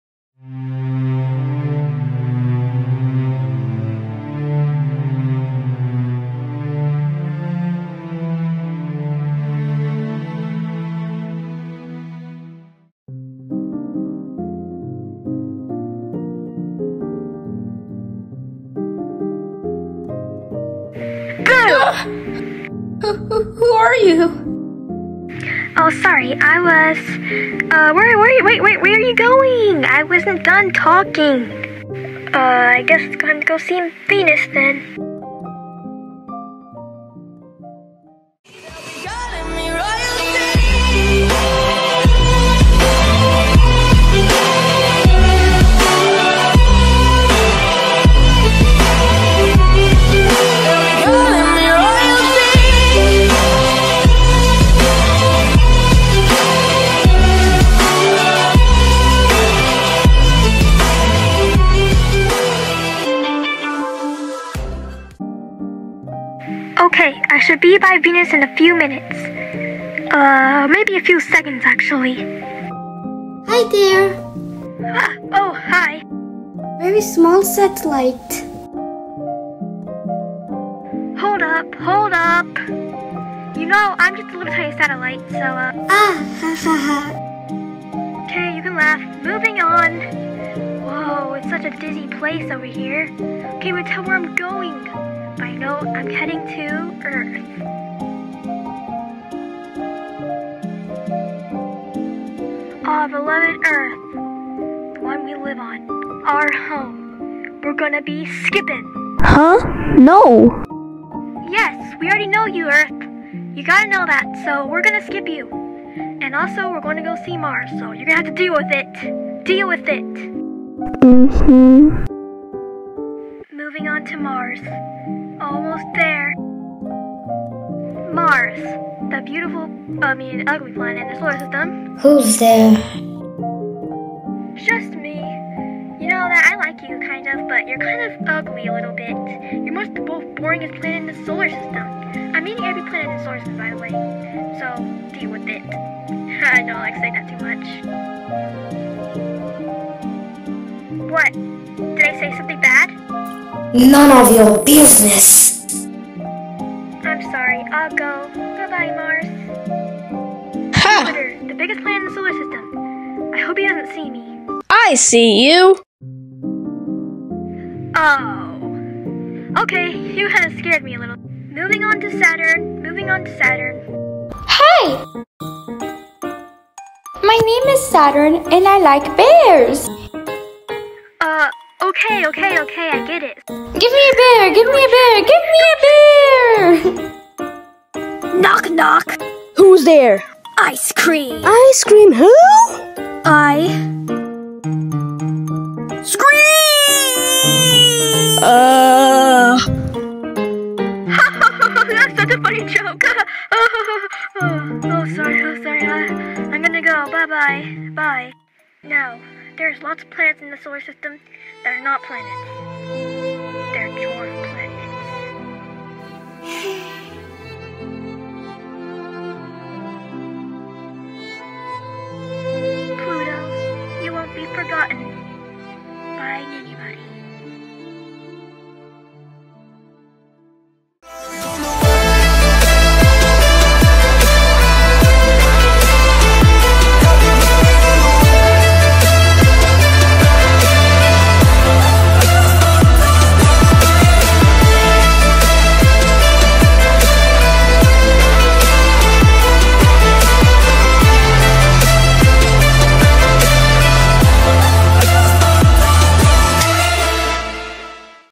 SolarPlanets || episode one || Meet the planets
Episode one! Finally done! My original series finally begun!

More info in the outro

Cast:
Mercury: @UniverseFacts5
Venus: @bellgium19
Earth: @totallynotvenus
Mars: @free_muslim_palestine
Jupiter: @JupiterTheGasGiant
Saturn: @ur._.localzyn
Uranus: @Urantum.
Neptune: @Lolmander
Satellite: @Urantum.
Pluto: !nobody yet!